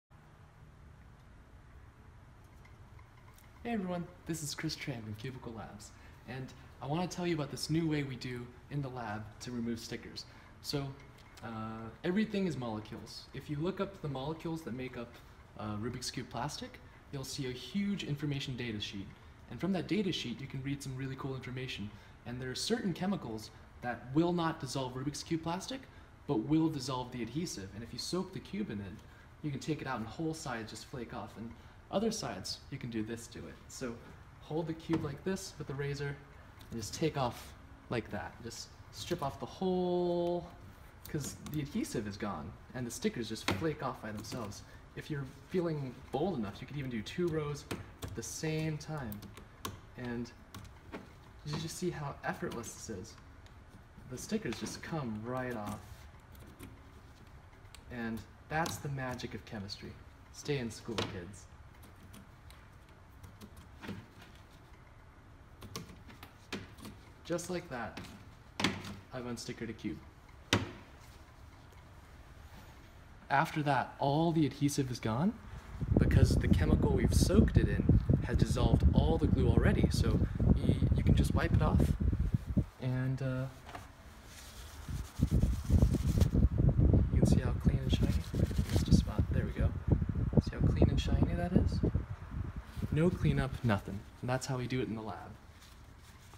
This is how Cubicle Labs unstickers hundreds of cubes in a day.
If you read materials data sheets for rubiks cube plastics, you can design a chemical mix that dissolves the adhesive without dissolving the plastic.

By using chemistry, we can solve real life problems.
Stay in school kids.